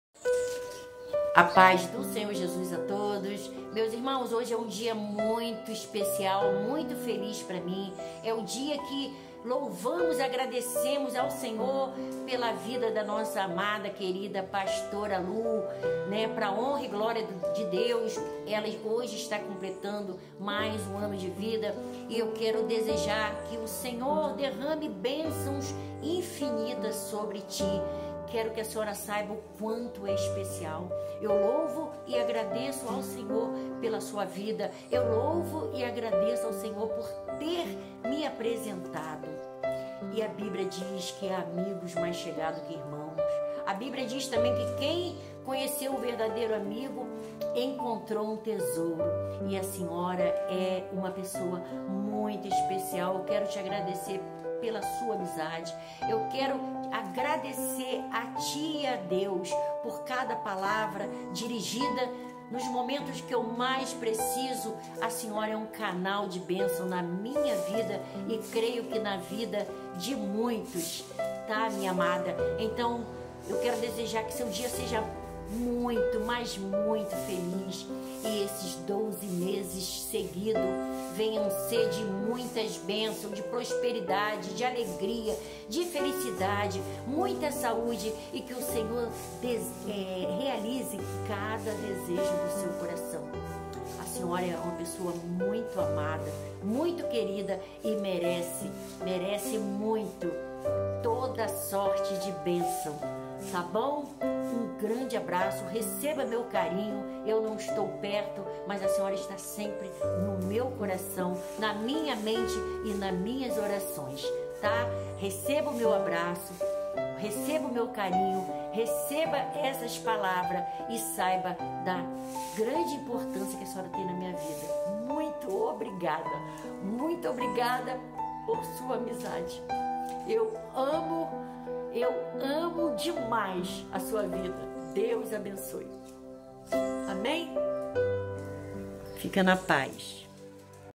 🥰 Feliz aniversário amada! @pastoralufeeesperanca Deus abençoe grandemente sua vida! 🙏🥰
A bíblia nos diz em Provérbios, 17:17. Em todo tempo ama o amigo; e na angústia nasce um irmão. Venho através dessa pequena homenagem te dizer o quanto é especial para mim, te desejo infinitas bençãos neste dia especial, muitos anos de vida, muitas felicidades, saúde e que Deus continue te usando como canal de bençãos em nossas vidas. Receba o meu carinho, minha simples homenagem de todo meu coração. Deus te abençoe grandemente, em nome de Jesus! Amém..❤️🌷🏵️💐